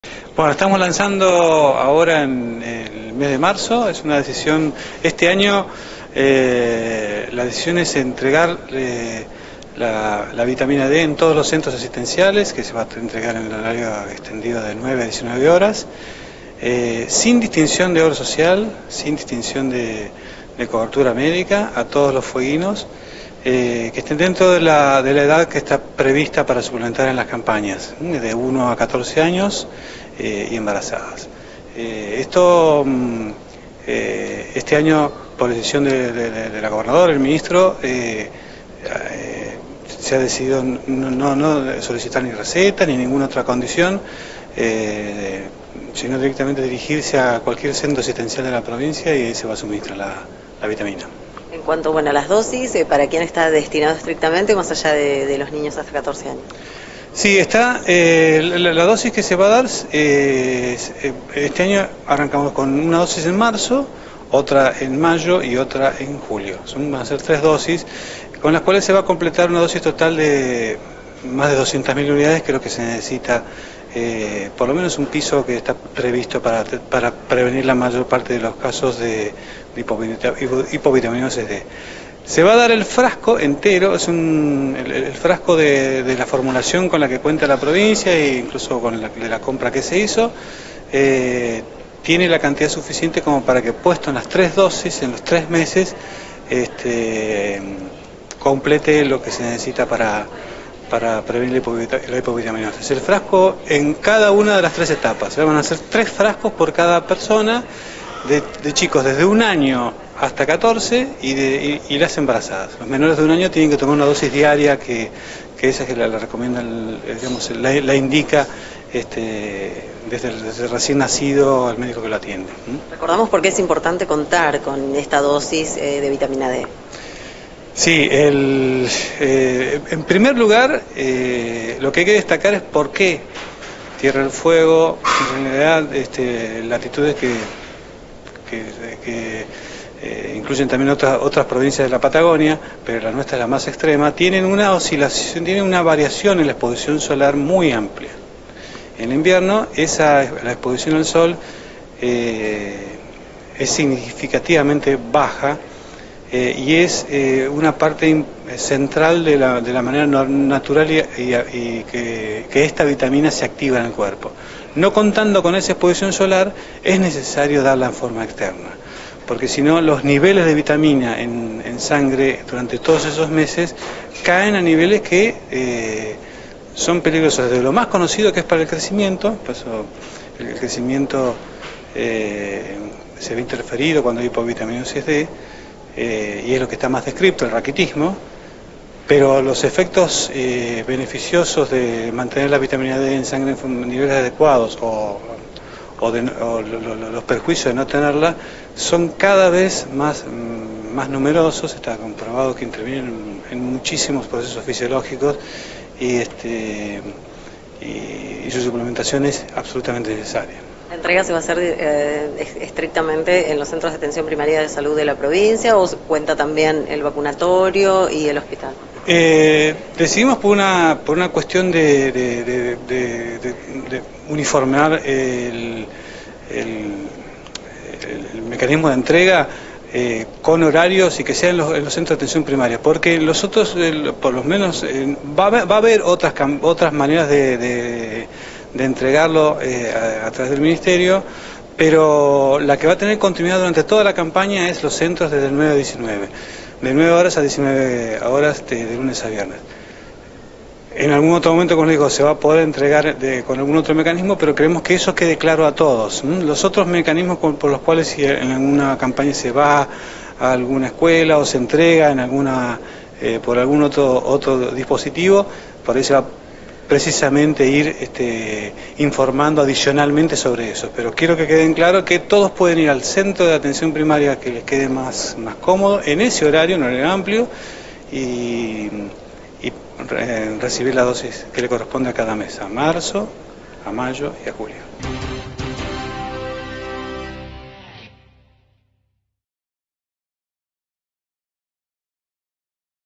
Vitamina D | Salud | 03/03/2017 - TV2 Noticias
La primera dosis se encuentra disponible en todos los CAPS y no se requiere presentar orden médica. Los niños/as de hasta 14años recibirán 3 dosis que se suministrarán en el mes de Marzo, Mayo y Junio.